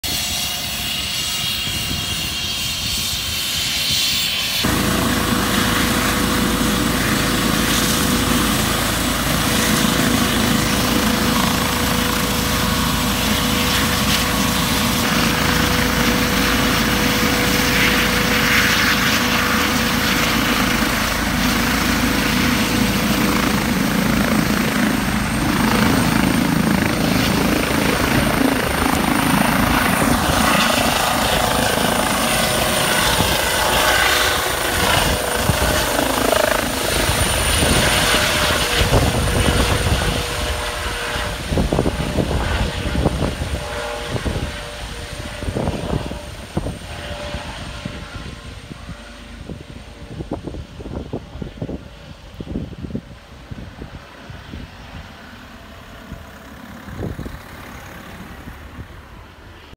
Hampshire & Isle of Wight Air Ambulance takes off from Brighstone on the Isle of Wight
The Hampshire and Isle of Wight Air Ambulance landed in the rural village of Brighstone in response to a medical emergency.